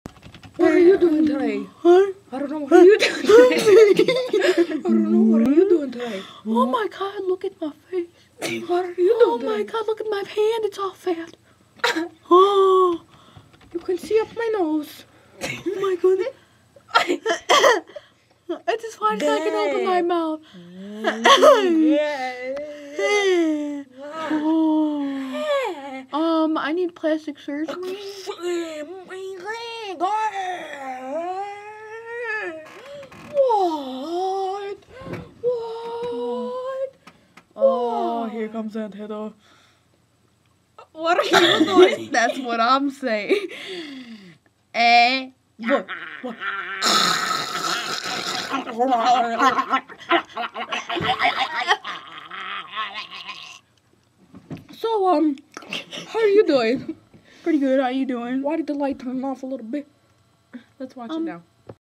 Mutants!!!
Bree, Savannah & Aunt Heather eat too much fish from Rogue River and MUTATE! Watch if you dare...